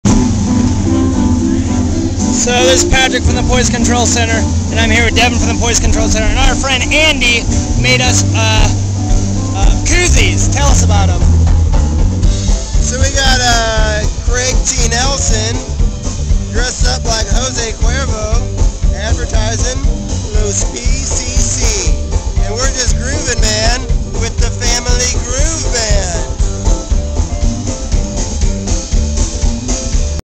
80/35: Poison Control Center
Patrick Fleming and other members of the Poison Control Center discuss their awesome new beer cozies.